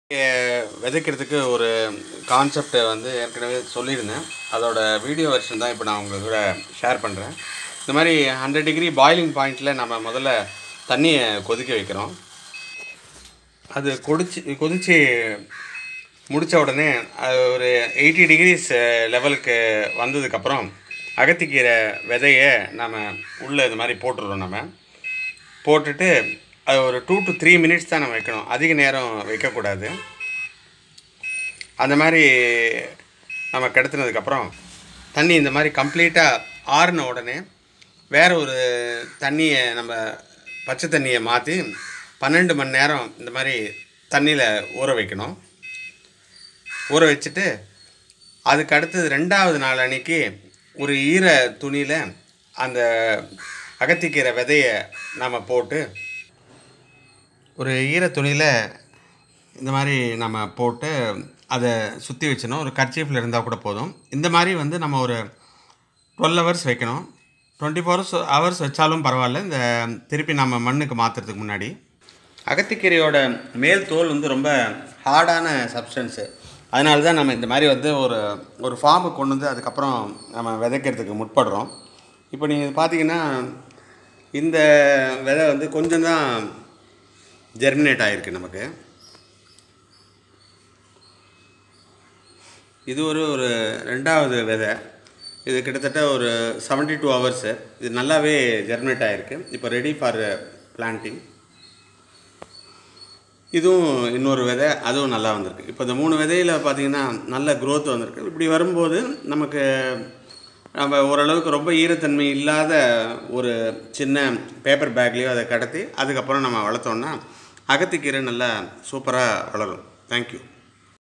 அகத்தி கீரை விதையை எப்படி விதைப்பது..How to sow agathi keerai
simple and powerful method to successfully grow agathi keerai.K. saikumar